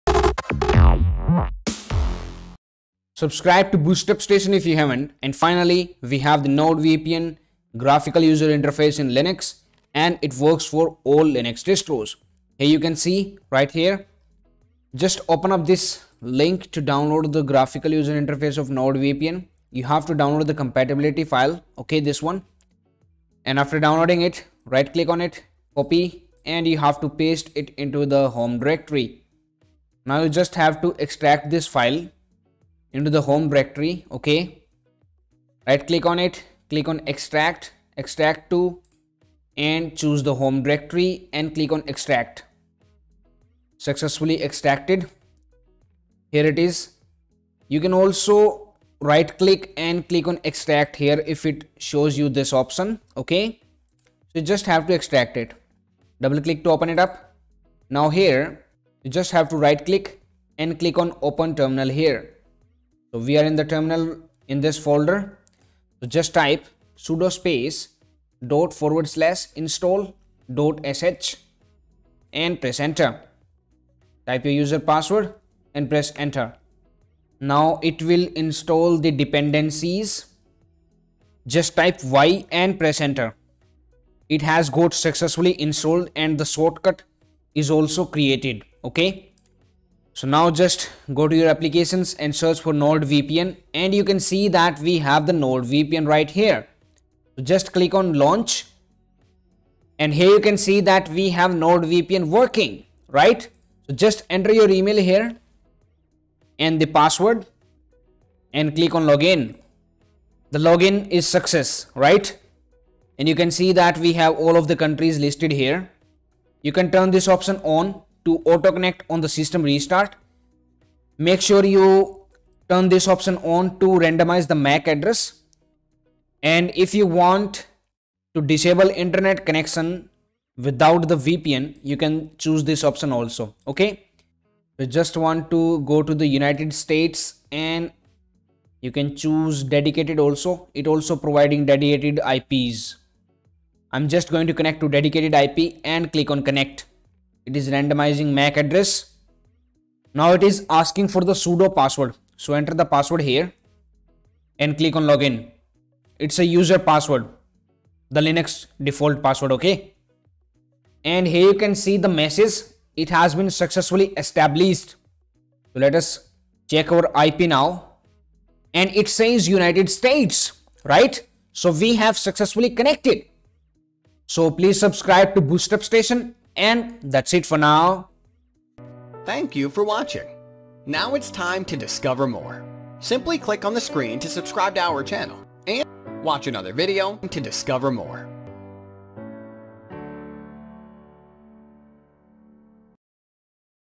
Install NordVPN app in Linux | NordVPN GUI client for Linux | NordVPN Network Manager GUI in Arch
Install NordVPN app in Linux | NordVPN GUI client for Linux | NordVPN Network Manager GUI in Arch, Ubuntu, Kali, Fedora, CentOS, Manjaro, Linux Mint, OpenSuse, Debian, and all other linux distros.

Run this command in terminal before using below beta build:-
Please delete your old config file:
rm ~/.nordnmconfigs/.configs/nord_settings.conf